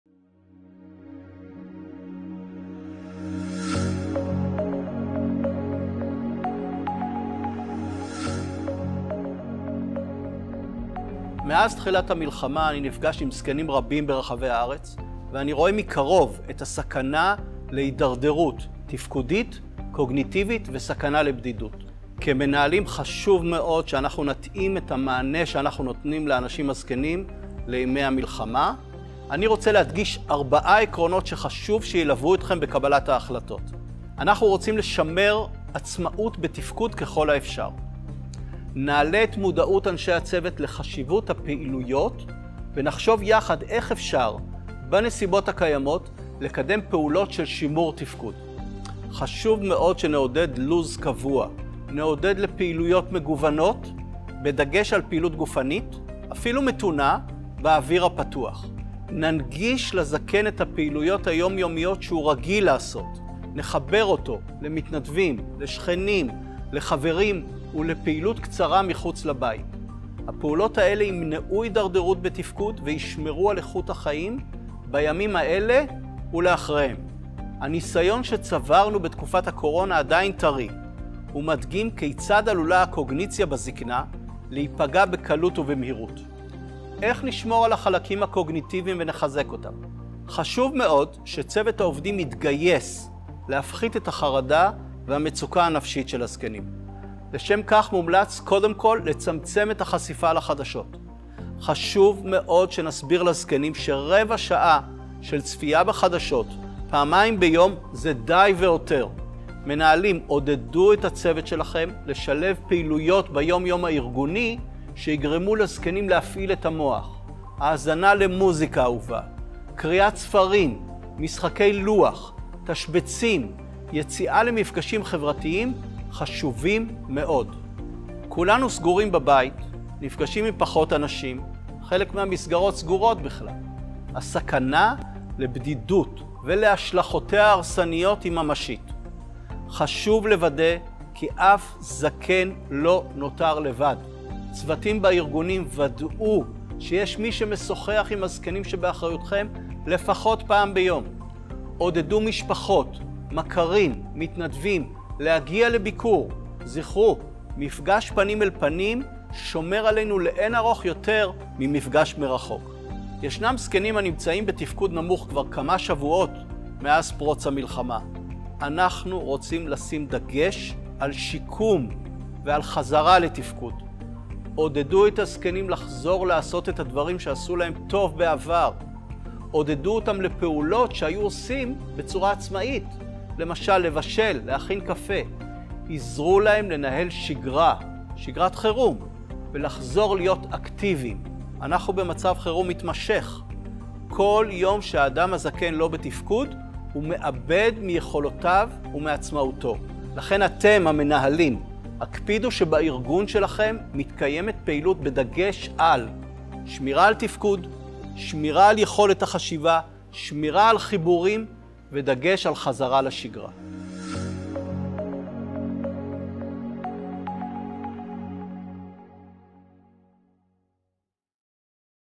זיקנה במלחמה | פרופ' יורם מערבי
כיצד נתאים את המענים שאנו נותנים לזקנים בימי מלחמה, תוך שמירה על תפקוד, קוגניציה, חיבורים ויצירת שגרה חדשה עבורם.
הסרטון מיועד למנהלי שירותים בזיקנה ולאנשי מקצוע בשטח בהנחיית פרופ' יורם מערבי, מומחה בגריאטריה, המרכזים הרפואיים הדסה-האוניברסיטה העברית, מנהל היחידה לשיקום בית ושיקום בקהילה, שירותי בריאות כללית, מחוז ירושלים.

הסרטון מוגש מטעם בית הספר לקידום הזדקנות מיטבית בשותפות עם משרד הבריאות, משרד הרווחה והביטחון החברתי, משרד הנגב הגליל והחוסן הלאומי, הביטוח הלאומי וג'וינט-אשל